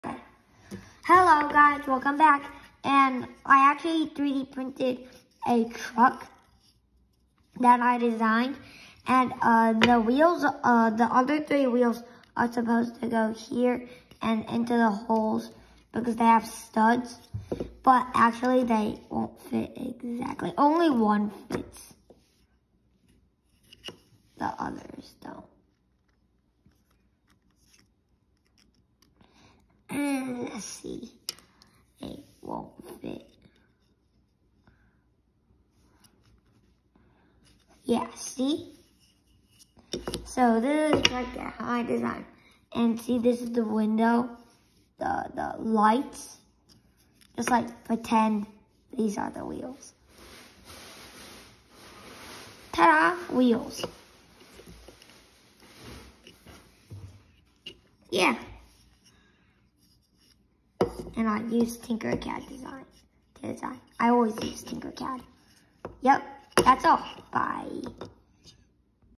3d printed truck
Zach's design, made some mistakes. He will improve next time.